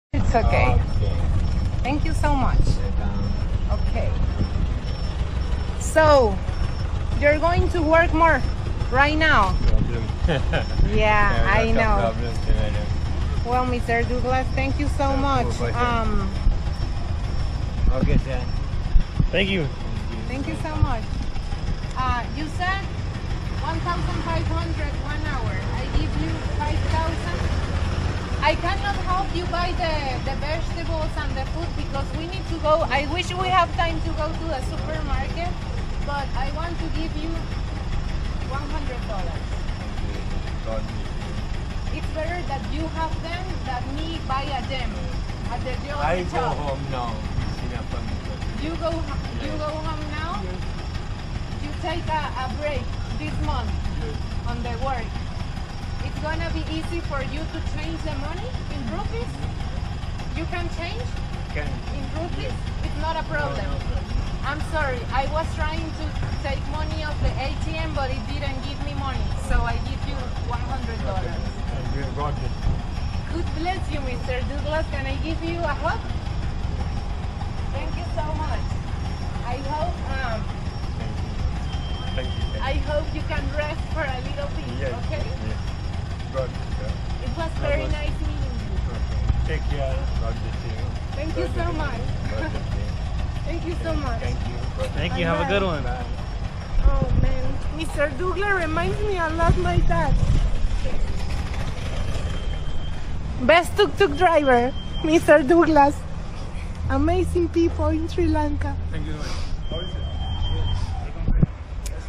විදේශිය කතකගේ හිත උනු විම
විදේශිය කතාවක් තමන්ව සුරක්ෂිතව නවාතැන් පලට ඇරලු ත්‍රිරෝද රථ රියදුරෙකුට ඩොලර් 100ක් ලබා දී ඔහුගේ ක්‍රියා කලාපය ගැන අගය කිරිමක් වර්ථා වේනවා